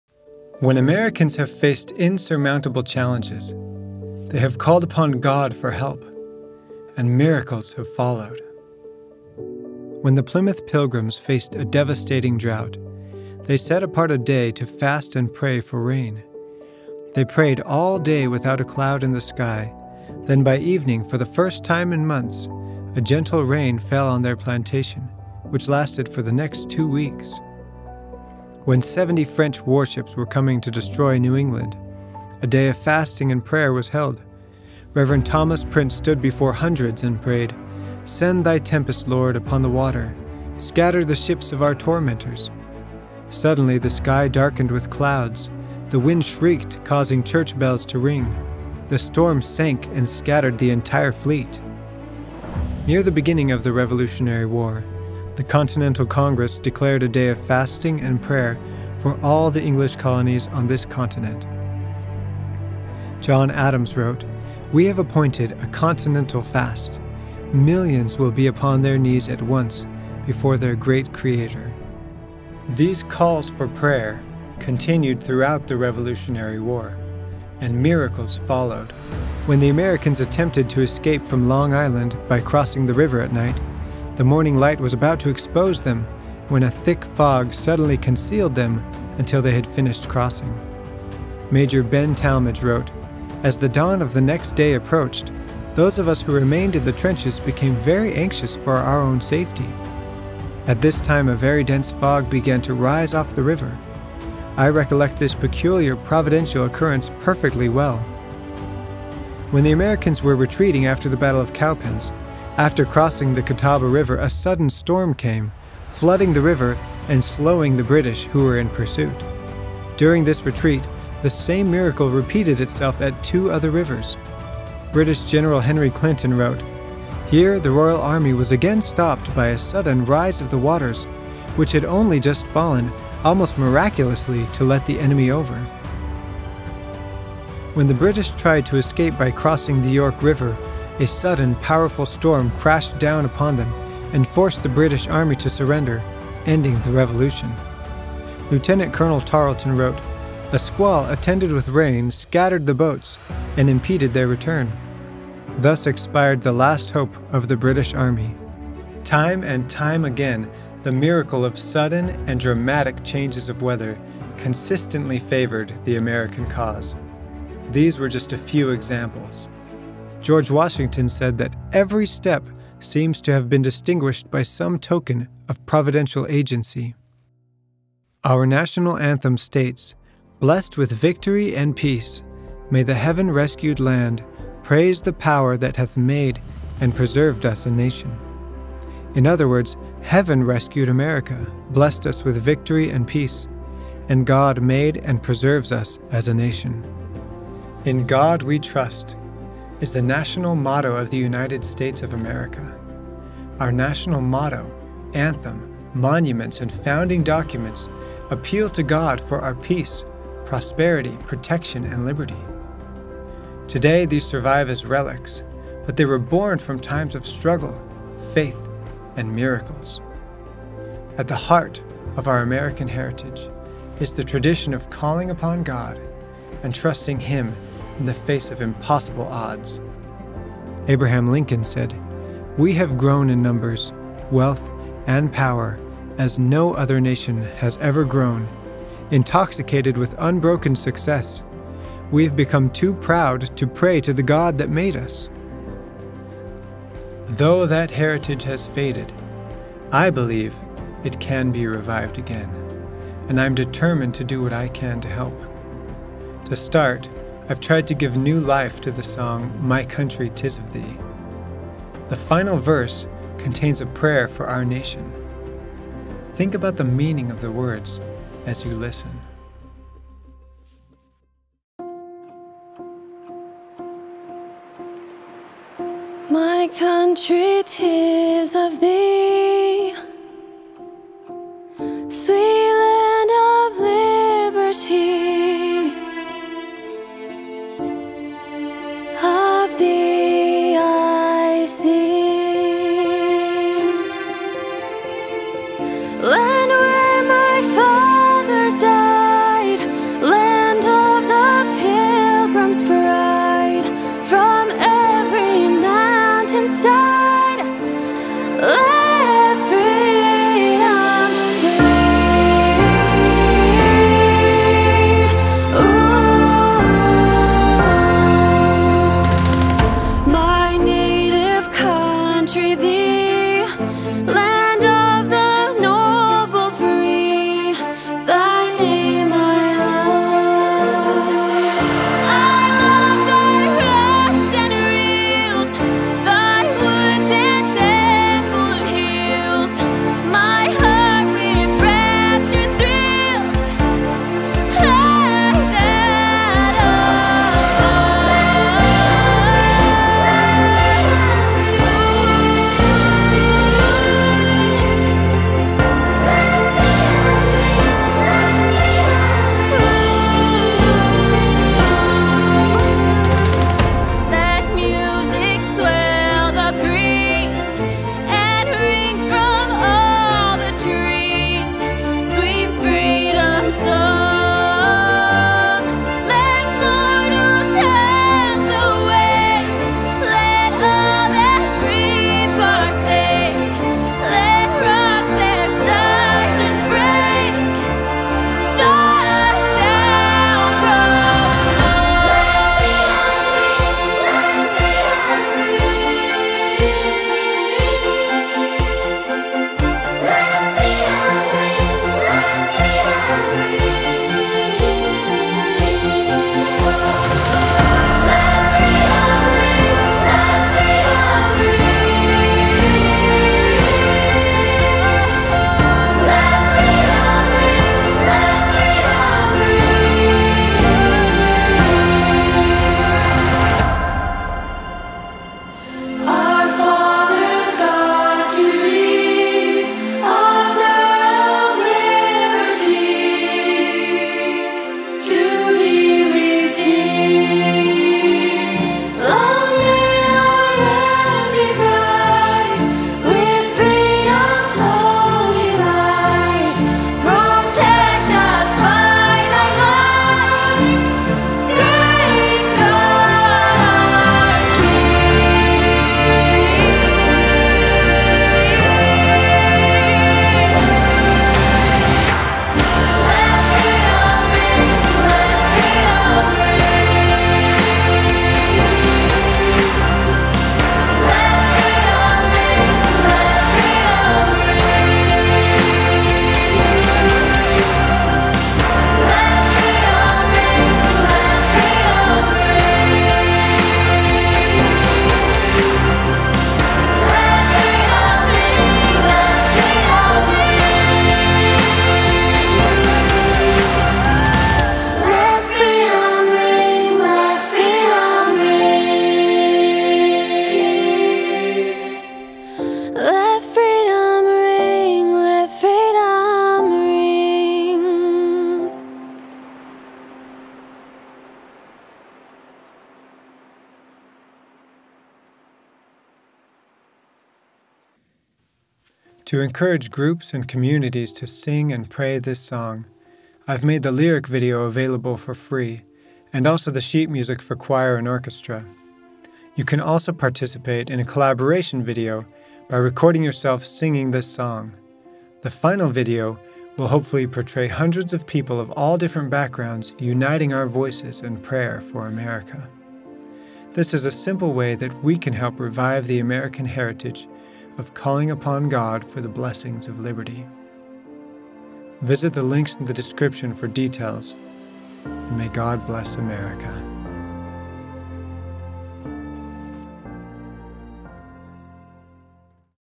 Pray for America: Reviving our American heritage of calling upon God for the blessings of liberty
My goal is to create uplifting and motivational music for people to enjoy around the world.